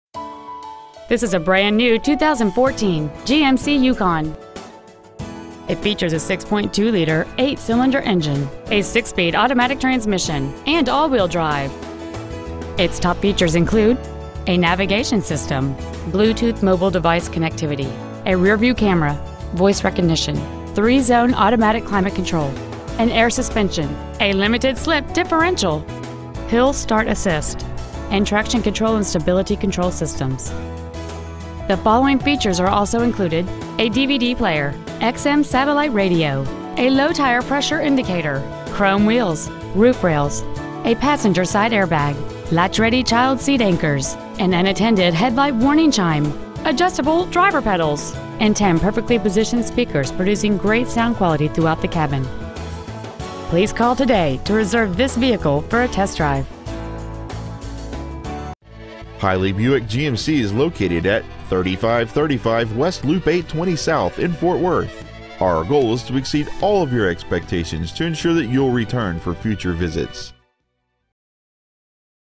2014 GMC Yukon XL Fort Worth TX 76116
We have been honored to serve the Fort Worth TX area , we promise that your experience at our dealership will exceed your expectations ! Year : 2014 Make : GMC Model : Yukon XL Engine : Gas/Ethanol V8 6.2L/378 Trans . : Automatic Exterior : White Diamond Tricoat Miles : 5 Interior : Light Cashmere Stock : G6271 Hiley Fort Worth Buick GMC Subaru W Loop 820 S Fort Worth , TX 76116